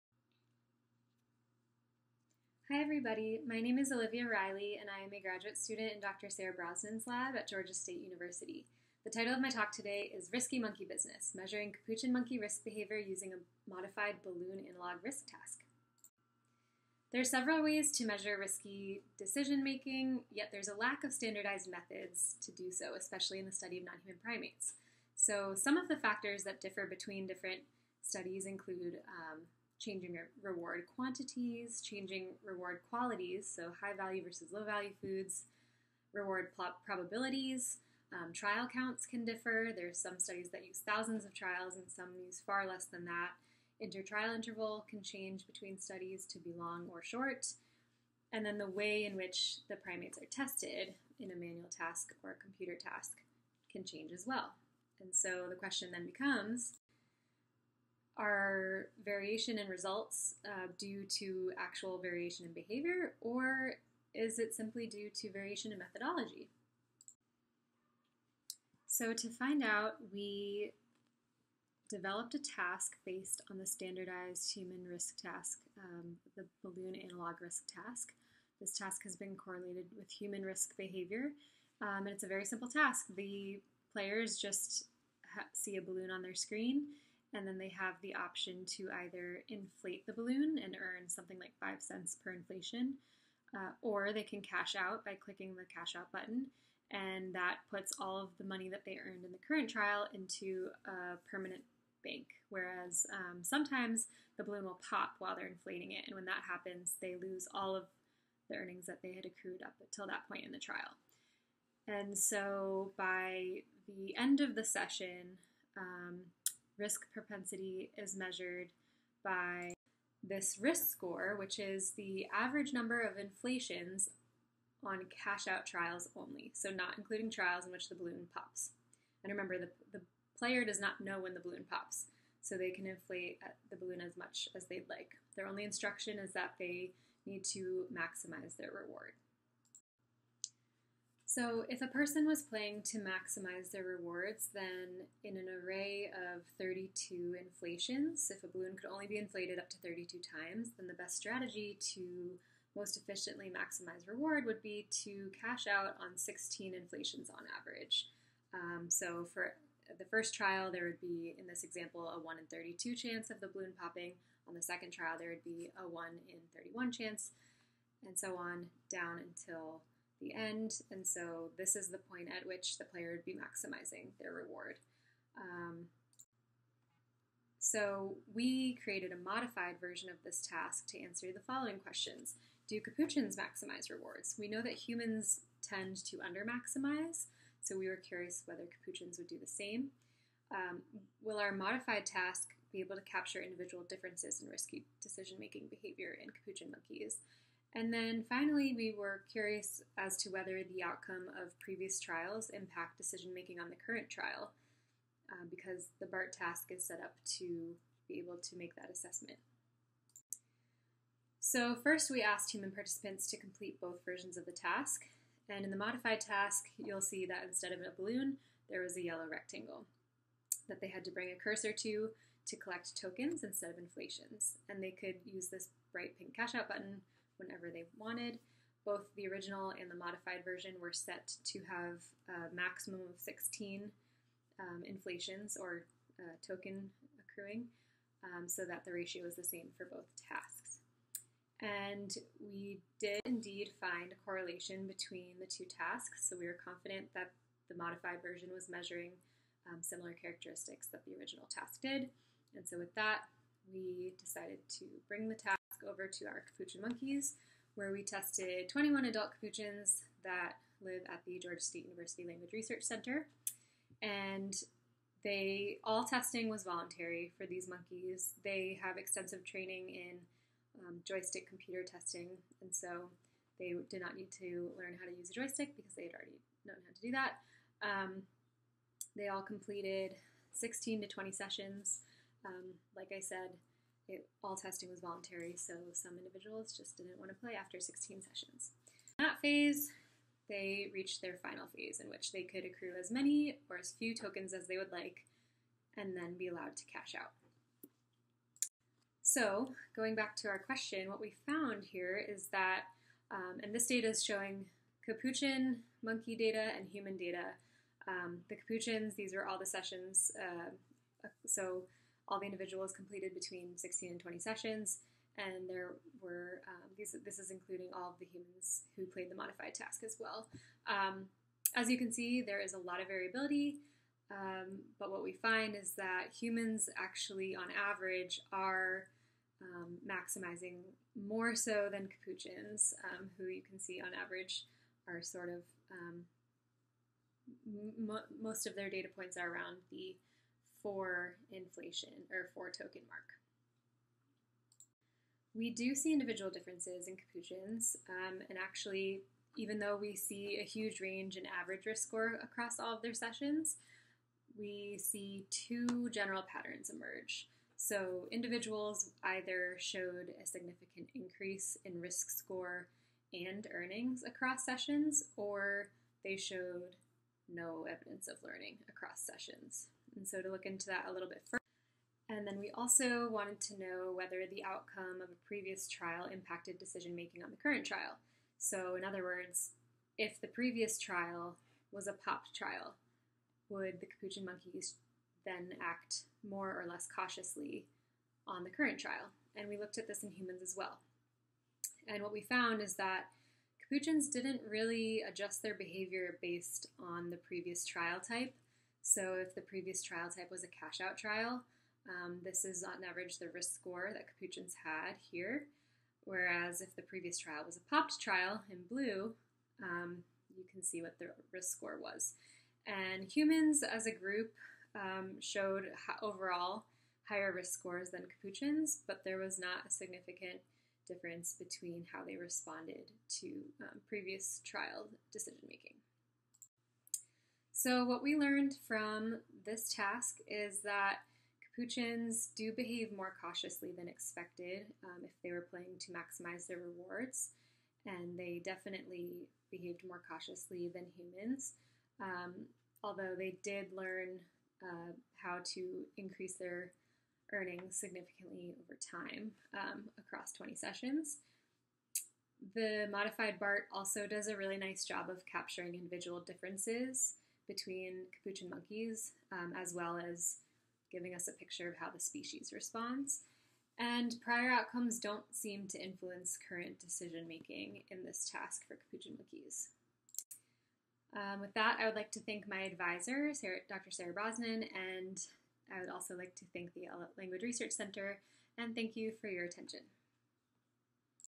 Risky monkey business: Measuring capuchin monkey risk behavior using a modified...[Olivia T. Reilly]
Animals are constantly faced with uncertainty in their environment and must make decisions in response to this uncertainty despite not always knowing what the outcome of their choices will be.
 Although there is an increasing amount of research on risky decision-making, there is significant variation in risk behavior across species and contexts, making comparisons difficult. This could
be due to the variety of methods used to assess risk. To address this, we created a modified version of the human risk assessment tool, the Balloon Analogue Risk Task (BART), to use for
 standardized assessment of risk behavior in human and nonhuman primates. We first verified that humans responded similarly to our adapted task as they would to the traditional BART. Then we
tested 21 capuchin monkeys on our modified BART program. Capuchins under-maximized their rewards, as humans who play the traditional BART tend to do. Capuchins behaved more cautiously
than humans did when playing the modified task, suggesting that capuchin monkeys are more risk averse than humans. This method allows us to directly compare risk behavior across species to
better understand how risky decision-making has evolved.